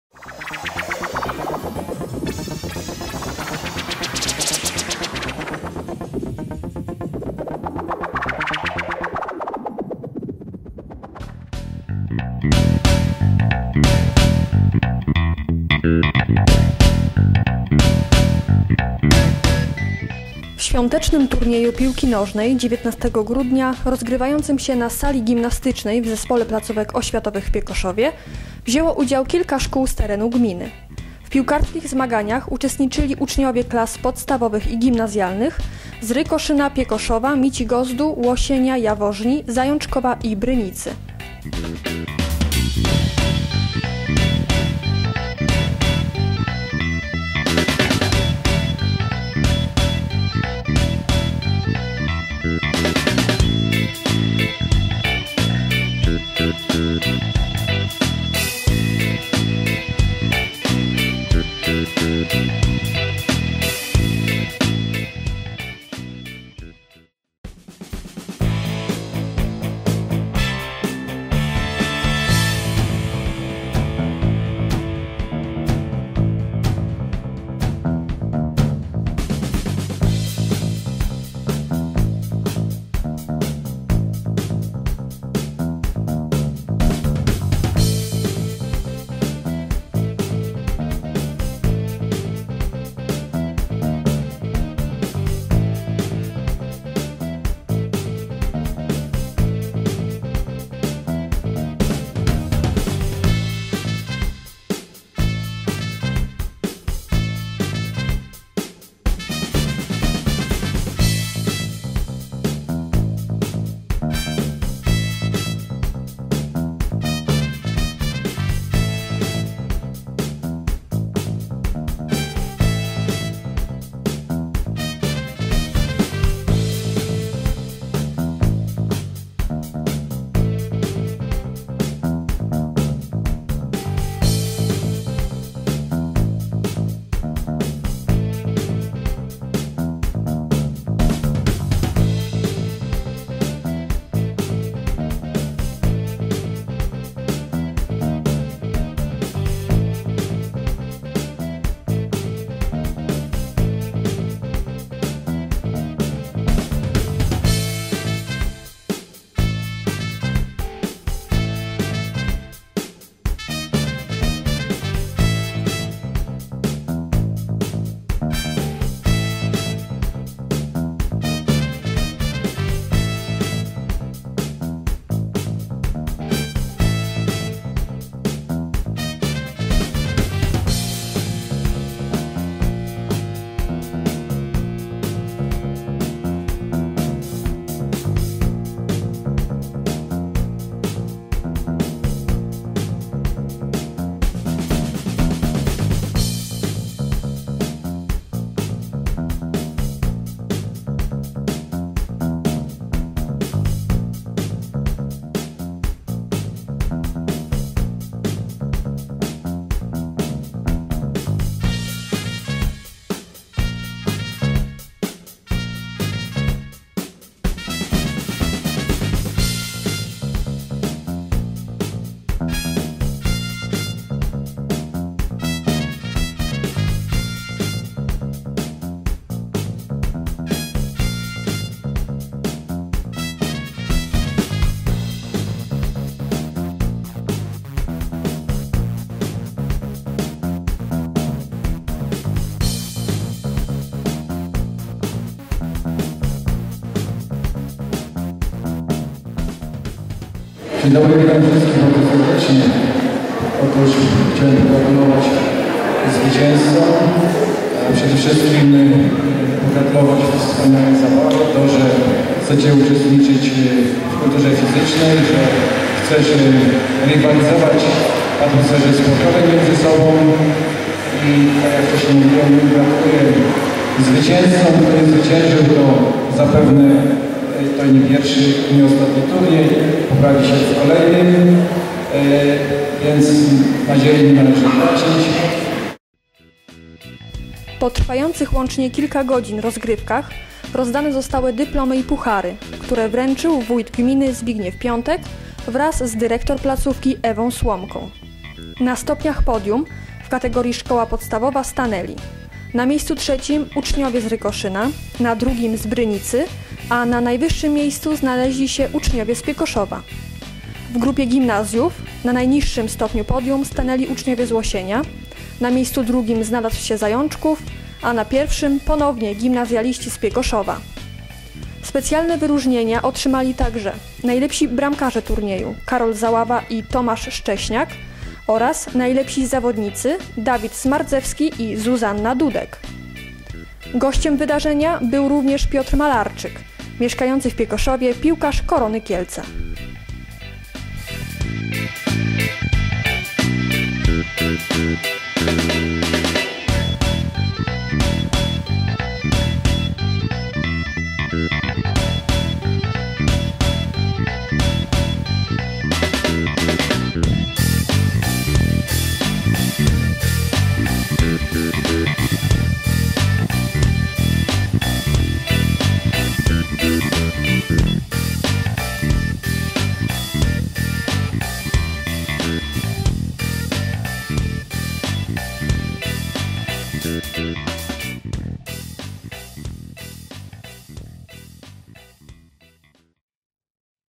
Świąteczny Turniej Piłki Nożnej - 19 grudnia 2013 ZPO Piekoszów
W Świątecznym Turnieju Piłki Nożnej,19 grudnia, rozgrywającym się na sali gimnastycznej w zespole placówek oświatowych w Piekoszowie, wzięło udział kilka szkół z terenu gminy. W piłkarskich zmaganiach uczestniczyli uczniowie klas podstawowych i gimnazjalnych z Rykoszyna, Piekoszowa, Micigozdu, Łosienia, Jaworzni, Zajączkowa i Brynicy. Po trwających łącznie kilka godzin rozgrywkach rozdane zostały dyplomy i puchary, które wręczył Wójt Gminy Zbigniew Piątek wraz z Dyrektor placówki Ewą Słomką. Gościem specjalnym Turnieju był Piotr Malarczyk.